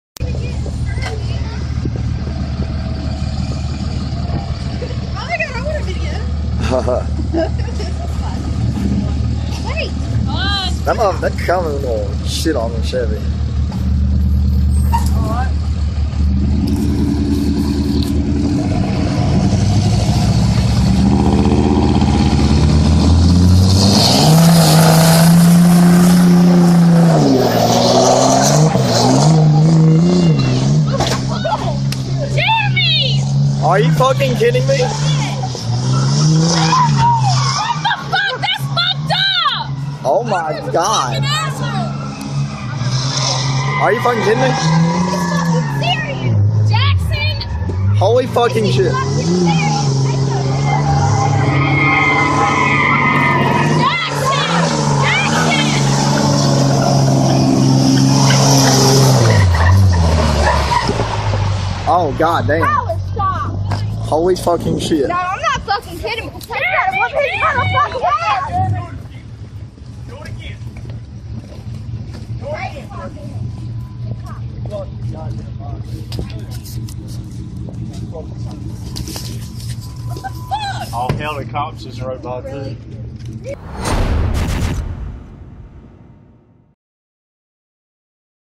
Dodge vs. Chevy Truck Tug o' War Humiliation
So... Dodge wins.